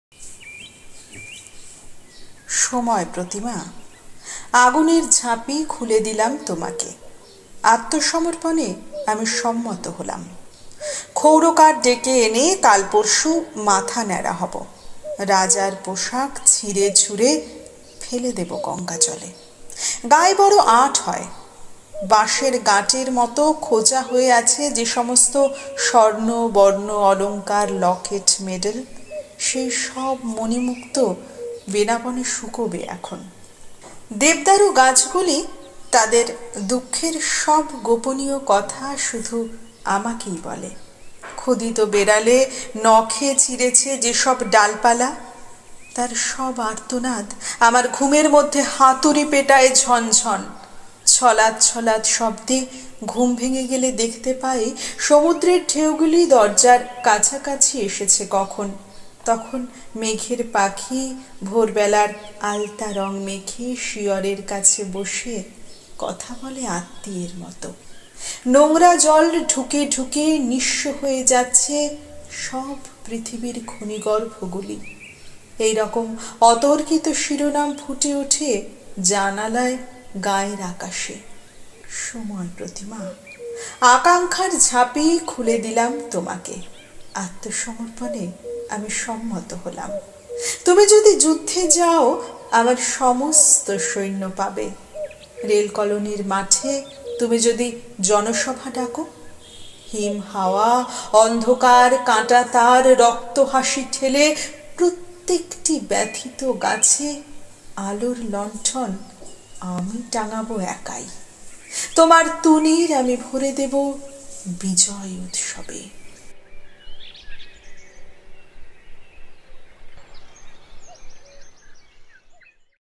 আগুনের ঝাঁপি|Amader Tumul hoi holla| পূর্ণেন্দু পত্রী| Purnendu Pattrea|Anindita Bhowmick
আমাদের তুমুল হৈ হল্লা (১৯৮০)সিরিজ

আগুনের ঝাঁপি
পূর্ণেন্দু পত্রী
কণ্ঠে - অনিন্দিতা ভৌমিক